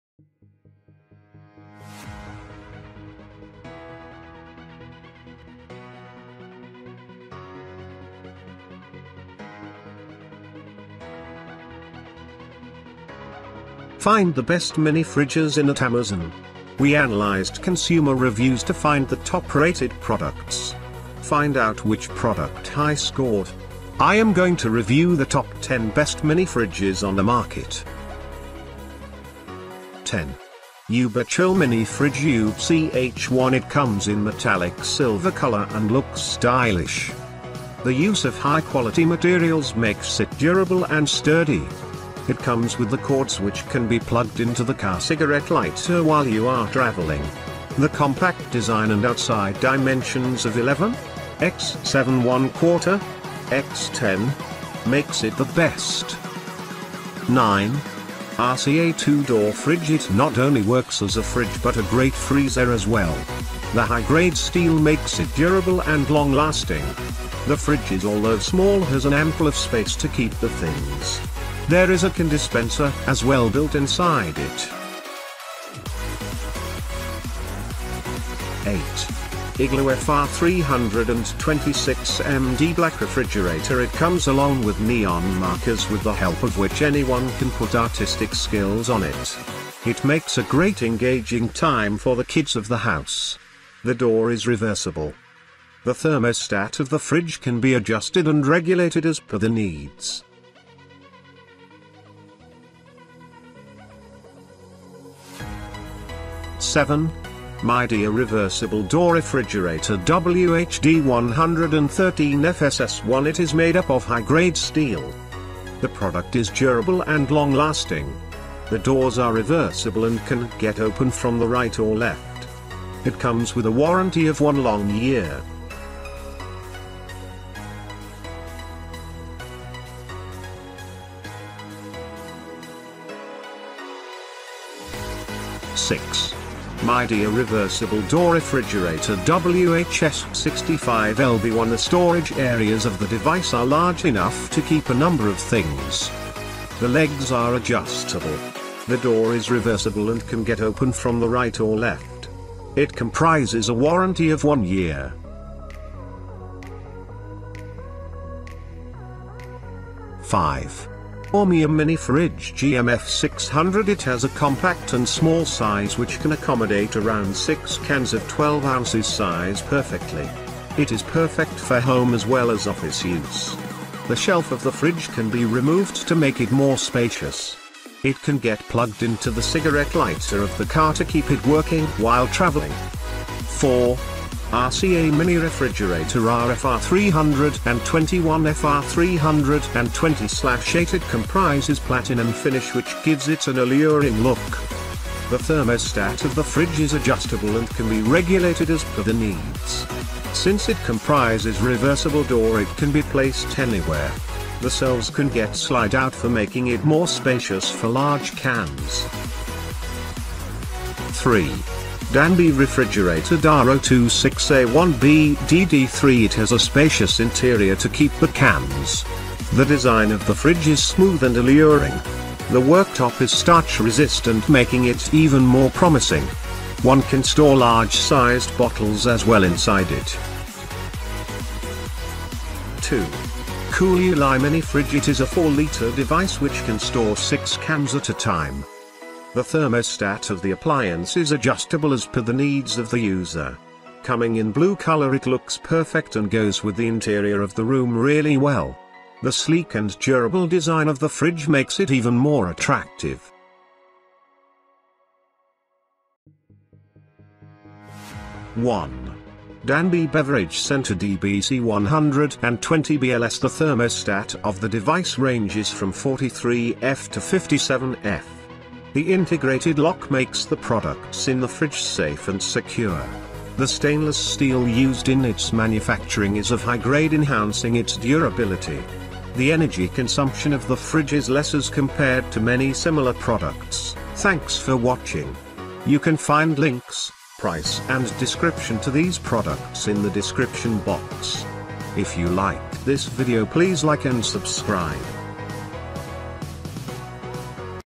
10 Best Mini Fridges of 2023
Hi guys, I am going to show video review on the top 10 Best Mini Fridges on the market. Get your Best Mini Fridges from here

Top 10 Best Mini Fridges